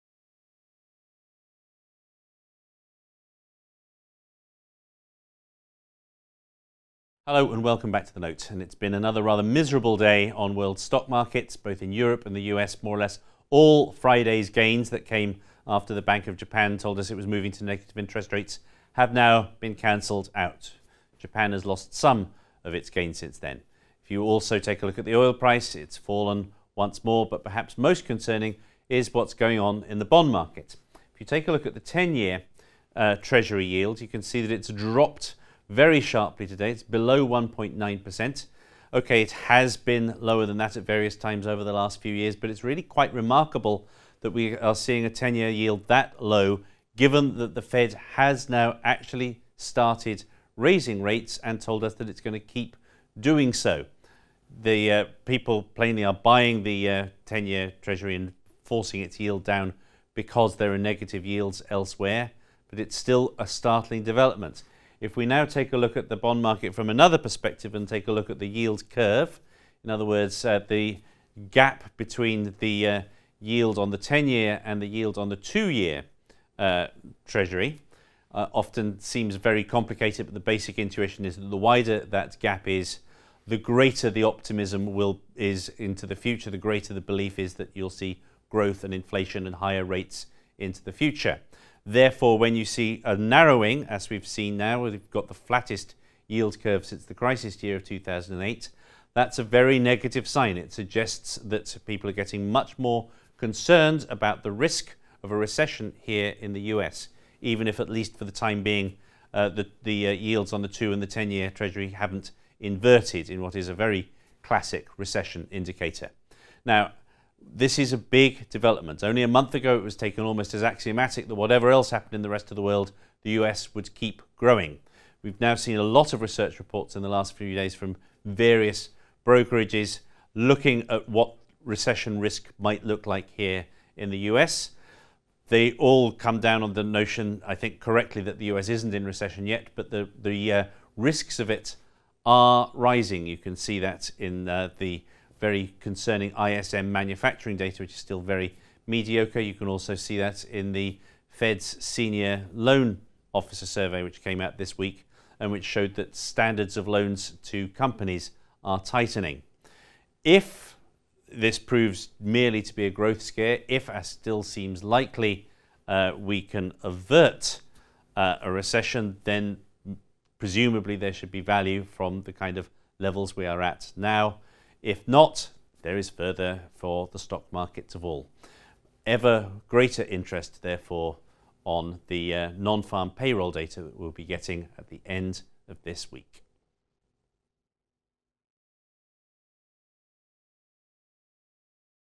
US recession risk | Authers' Note
John Authers examines a surge of money into US Treasury bonds and asks if it portends a recession.